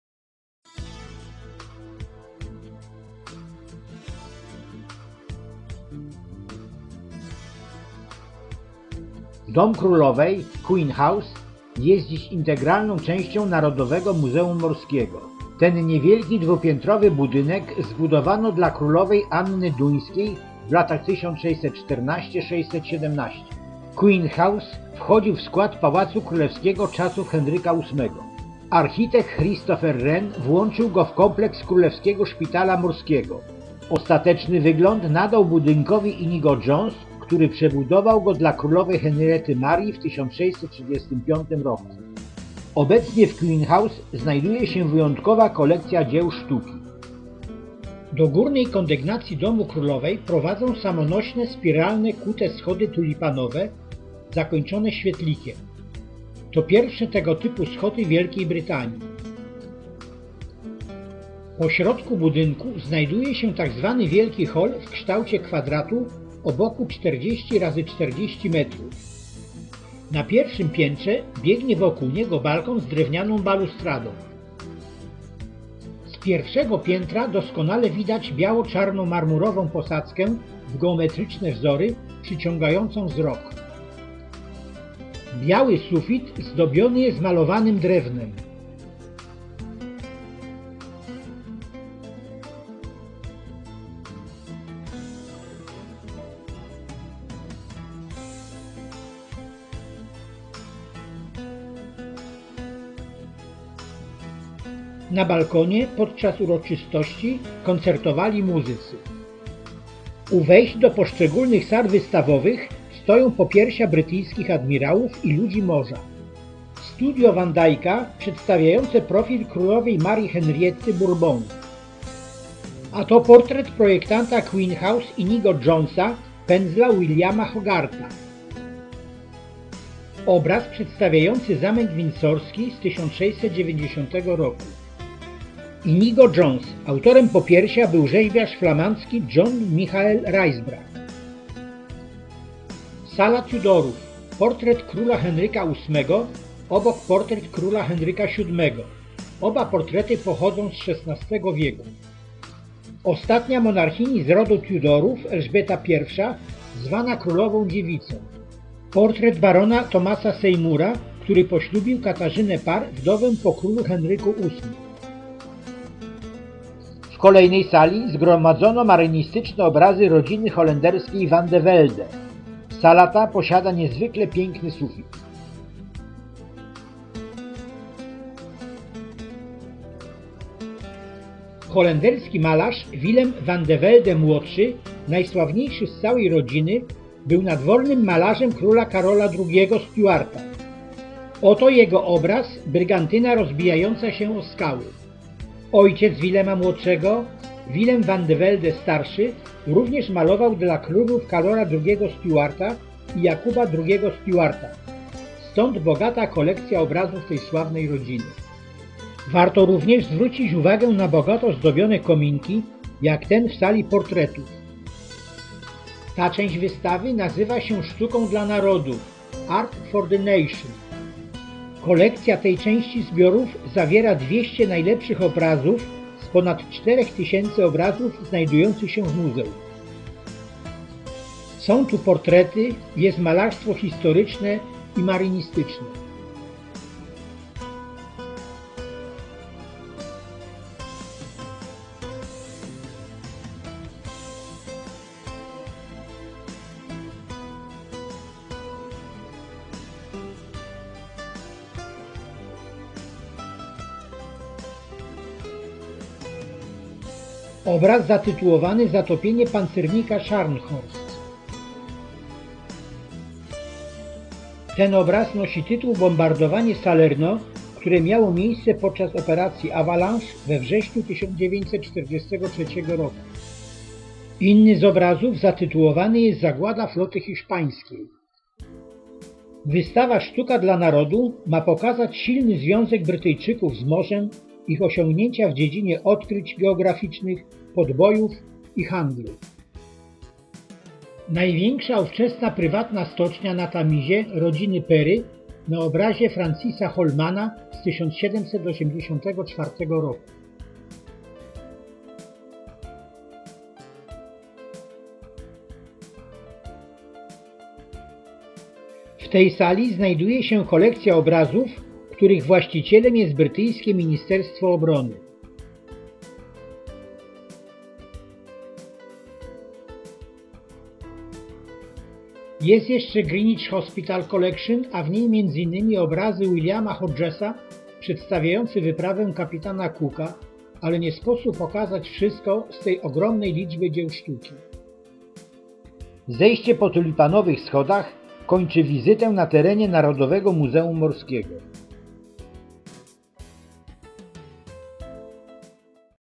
Londyn Greenwich. Dom Królowej.Queen House.HD
Była rezydencja królewska, wybudowana w latach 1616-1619 w Greenwich - Dom Królowej jest częścią składową Narodowego Muzeum Morskiego. Mieści się w nim znacząca kolekcja portretów i malarstwa marynistycznego.